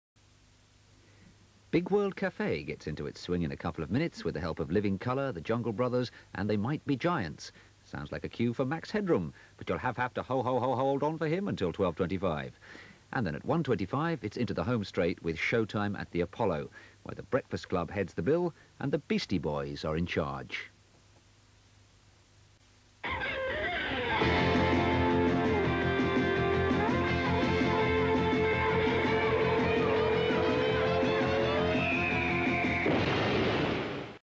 C4 / Channel 4 junction - 1989
The 11:25pm junction from 16th March 1989, with an eardrum-bursting version of the classic ident.

©️Channel Four Television Corporation
Any other appropriate copyright can be added on request.